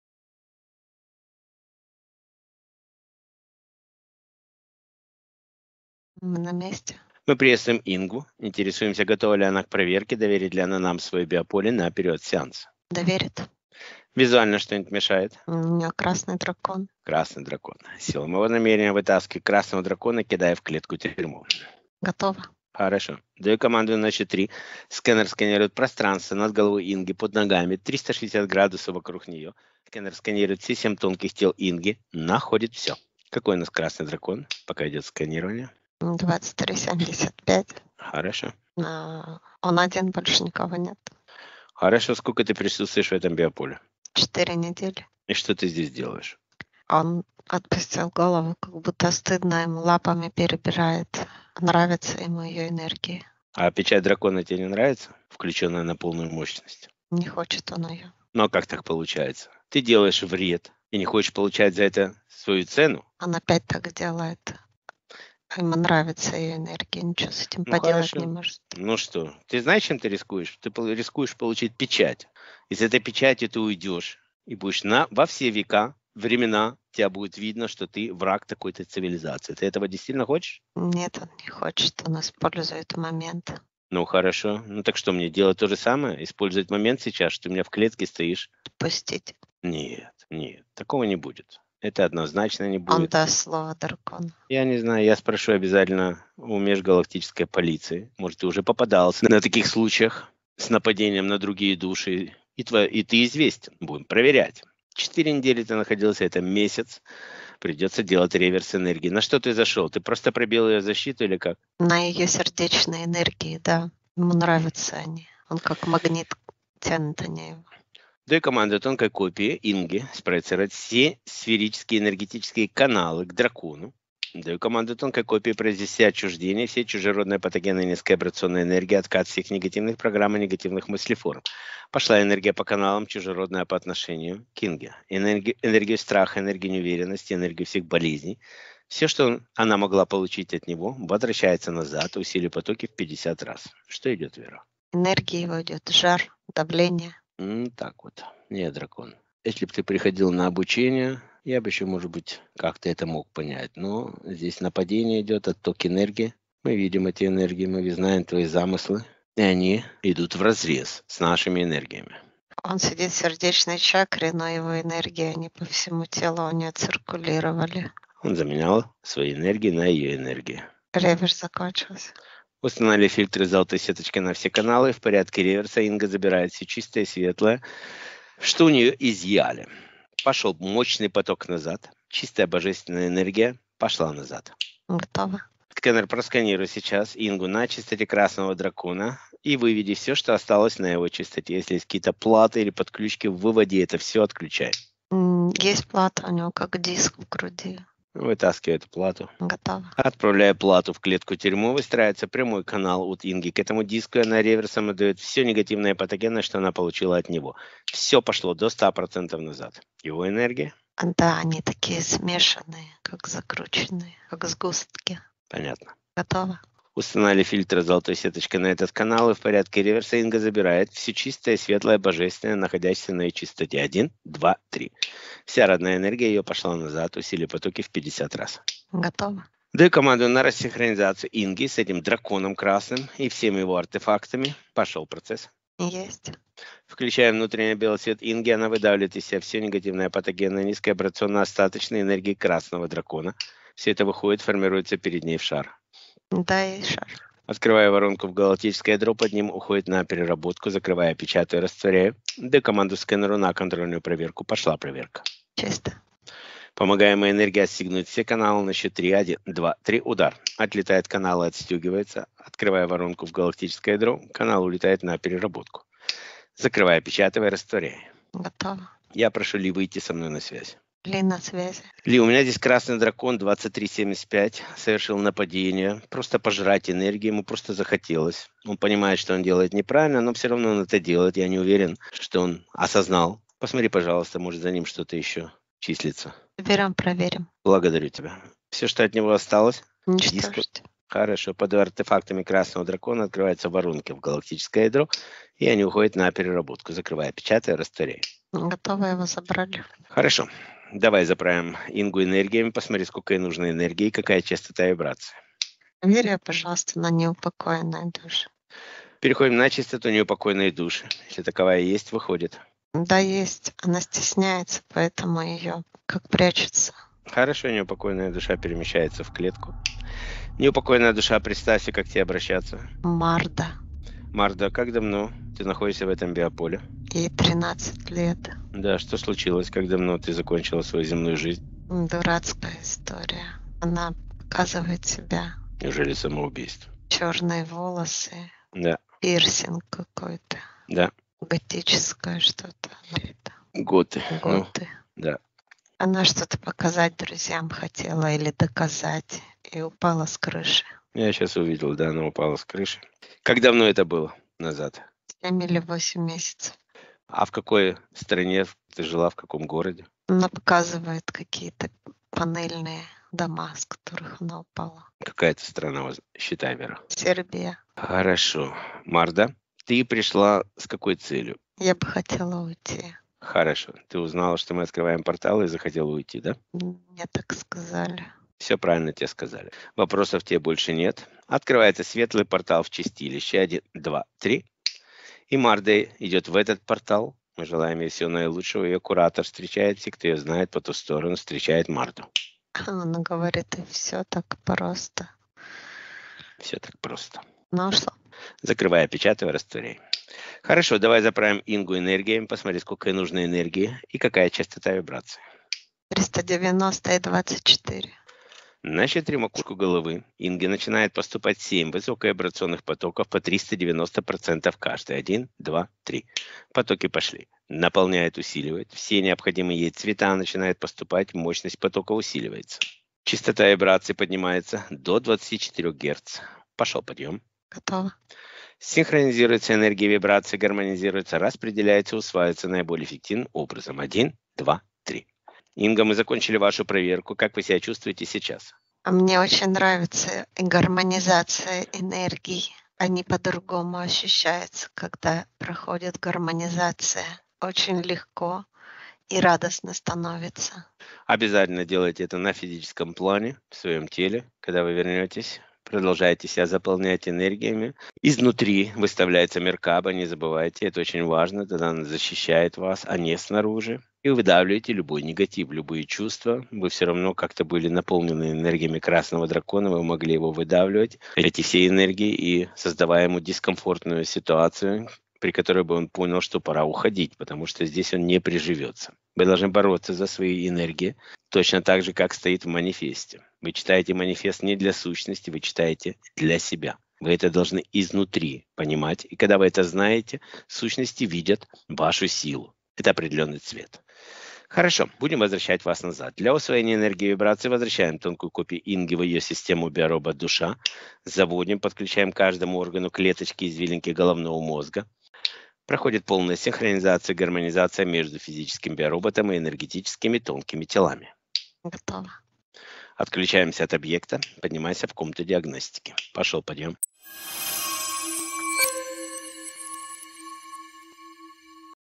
Нападение красного дракона | Портал для неупокоенной души
✅ По вопросам сеансов - администратор Ольга Нападение красного дракона | Портал для неупокоенной души | @ARGOVP2
00:00 Красный Дракон
02:00 Реверс энергий
03:07 Плата как диск в груди
04:30 Ли на связи
05:02 Неупокоенная душа
06:48 Заправка энергиями
07:15 Мы закончили проверку
08:15 Возвращаем тонкую копию